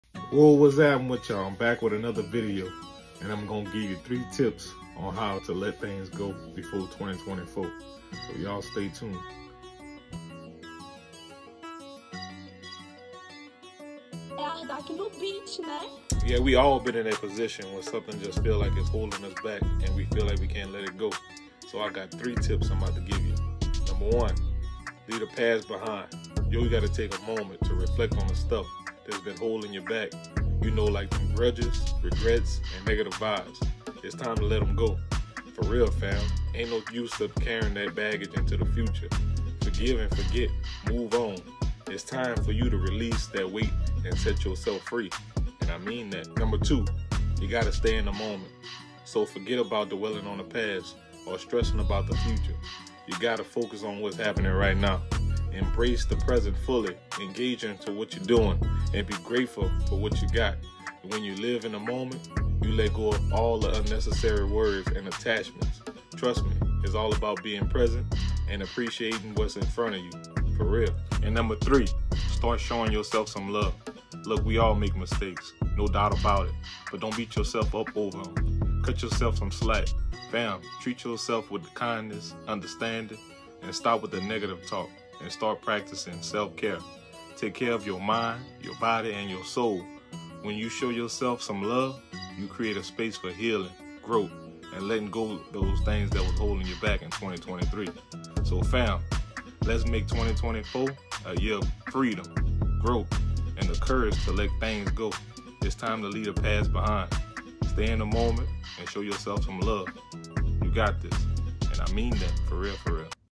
Letting Things Go Before 2024💪🏿
In this inspiring video, we're diving deep into the art of letting go and embracing new beginnings before 2024.

Life is a constant journey of growth, and sometimes we find ourselves holding onto things that no longer serve us. Whether it's past relationships, negative thoughts, or limiting beliefs, it's time to release them and make space for positive changes in our lives.

Join us as we share practical tips, powerful insights, and motivational stories to help you let go of anything that's been holding you back. We'll explore various techniques, such as journaling, meditation, and visualization, that can empower you to shed the old and step into a brighter future.

By using the hashtags Motivation, NewBeginnings, and SelfImprovement, we're determined to reach as many like-minded individuals as possible. We want to create a supportive community that uplifts and encourages one another on this transformative journey.

Don't miss out on this opportunity to gain valuable insights and gain the motivation needed to make positive changes in your life. Remember, 2024 is just around the corner, so let's make the most of it together!